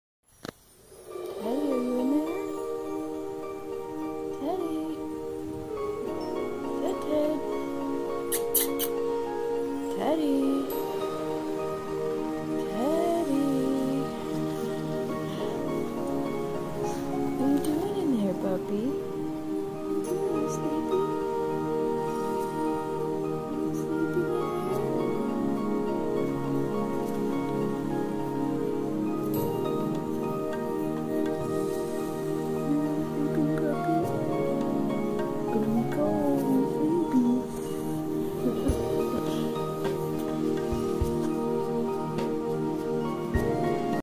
Teddy doesn't like the AZ cold.
Our first real cold morning of the season (40 degrees) & Teddy is hunkered down under a blanket. Unperturbed by my coming and going, he doesn't move from his "den". Usually he follows us everywhere, I guess you could say he's cold intolerant. Teddy will not tolerate cold.